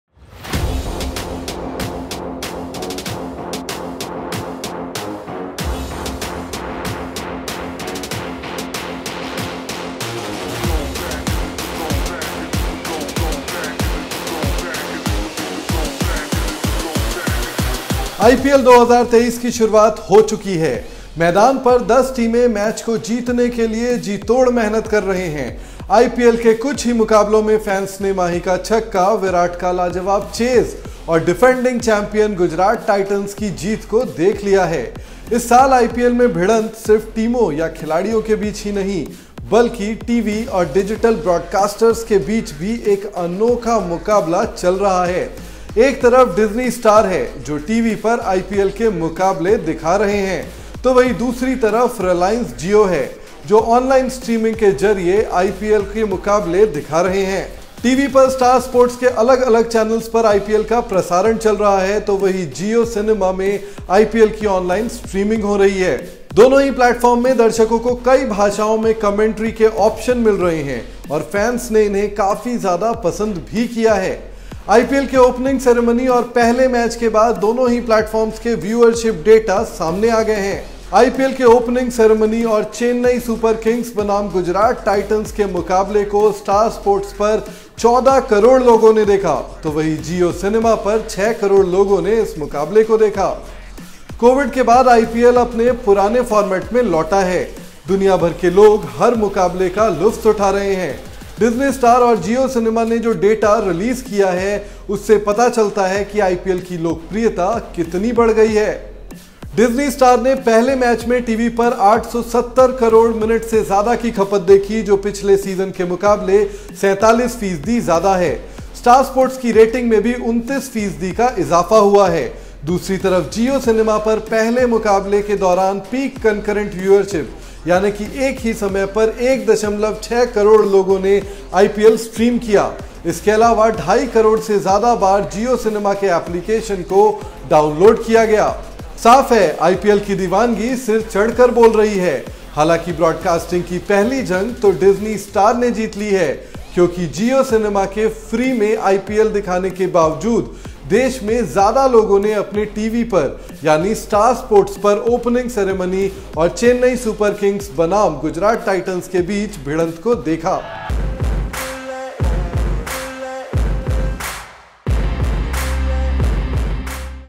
IPL 2023 में Star और Jio की बीच Viewership की जंग, पहले मैच के बाद कौन बना Champion? | Cricket News
Cricket News | IPL 2023 के पहले मैच में JIO Cinema  ने लगाई Viewership रिकॉर्ड्स की झड़ी
पहली बार प्रसारण कर रहे JIO Cinema ने IPL 2023 के पहले ही दिन CSK और Gujrat Titans  के बीच खेले गए मुकाबले के दौरान viewership के कई नए रिकॉर्ड कायम किए।